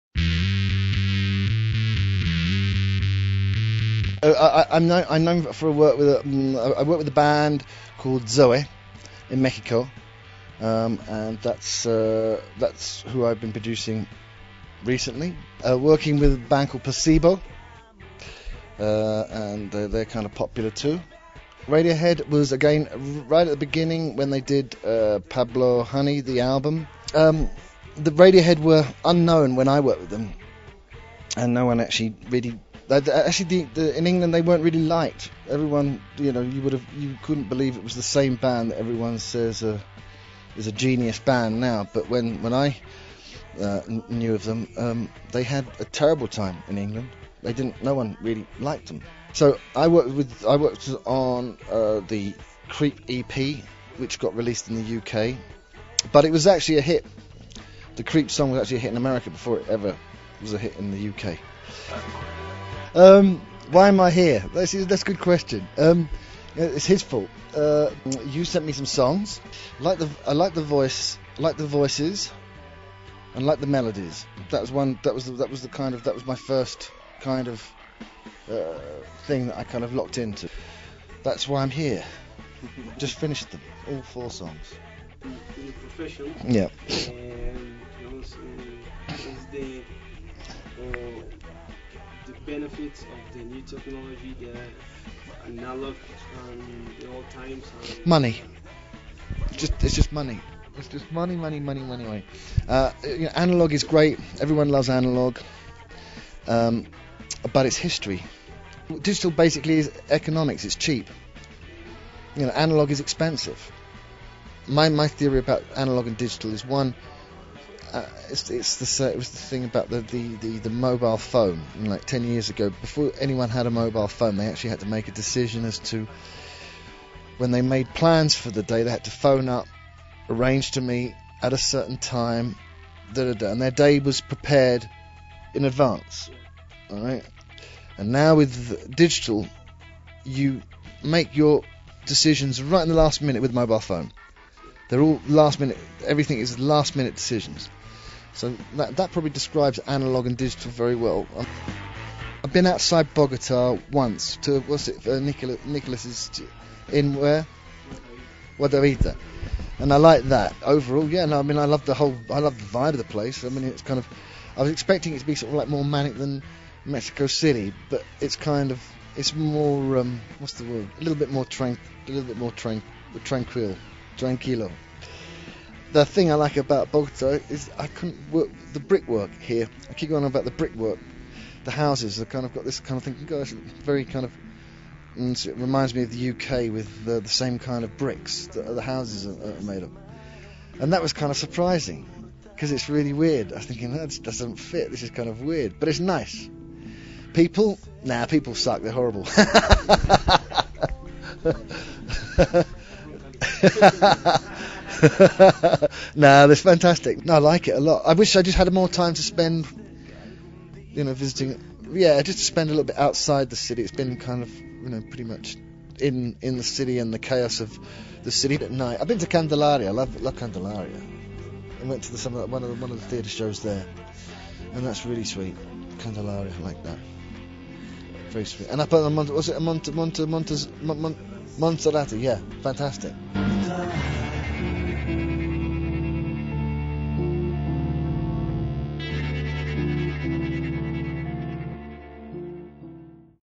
Phil Vinall
El productor de Zoé, Placebo y Radiohead habló con vive.in en su breve visita a Colombia